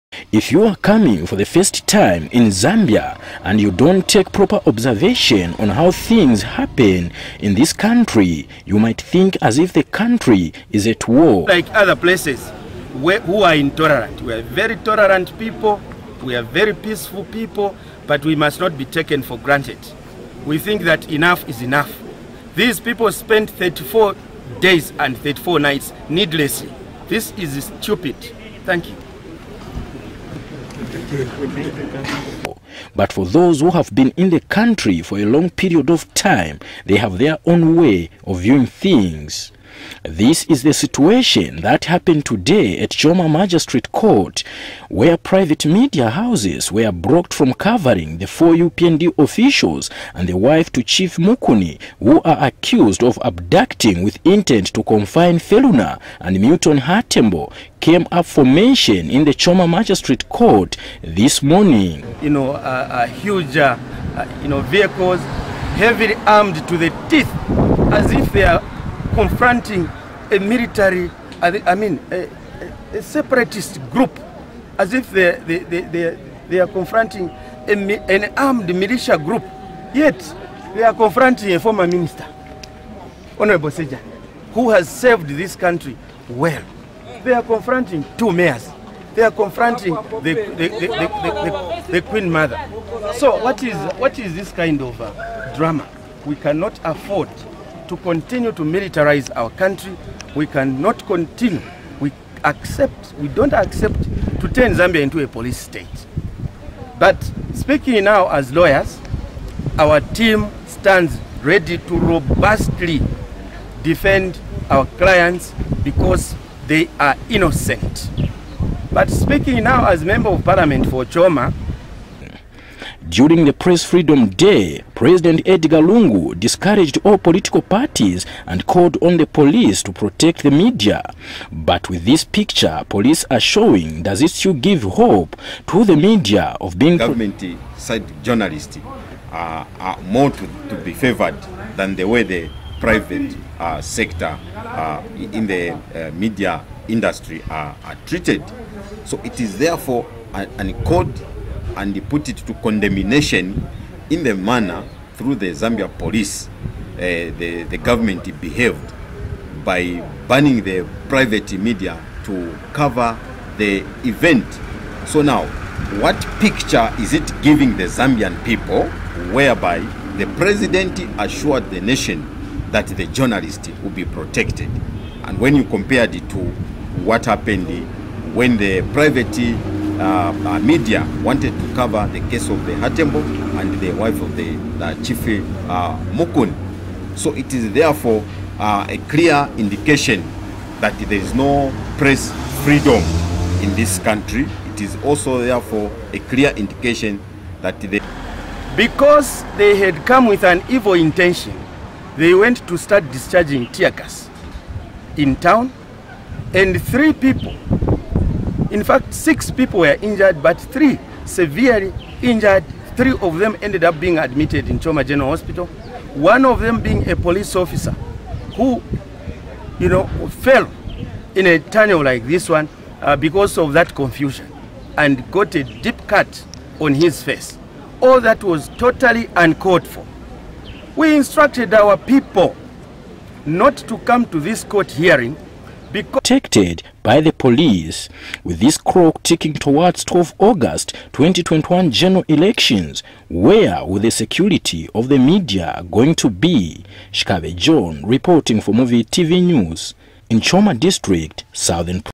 The Hatembo Abduction Case Attracts A Huge Presence Of Police In Choma
The case in which Choma business man Fines Malambo, four UPND officials and Wife to Chief Mukuni Veronica are accused of abducting with intent to confine Pheluna and Milton Hatembo came up for mention in the Choma magistrate court on Thursday morning.

The case had attracted a huge presence of the Zamia Police Service.

The development prompted lead defense Counsel, Martha Mushipe made an application to seek ruling on the heavy presence of the police officers at the court grounds.

Ms. Mushipe charge that the presence of the police is making it difficult for the accused relatives and the media have access to court.

In response, state Advocate Chitengi Kahilu says its a constitutional mandate of the Zambia police to provide security in the country.

But Senior Resident Magistrate, Peter Mungala has reserved the ruling to the next court sitting on 26th of May, 2021 for ruling and commencement of trial before Magistrate Idah Mupemo.

Meanwhile, UPND spokesperson, Cornelius Mweetwa has accused Zambia Police of looking for a fault against the innocent people of Choma.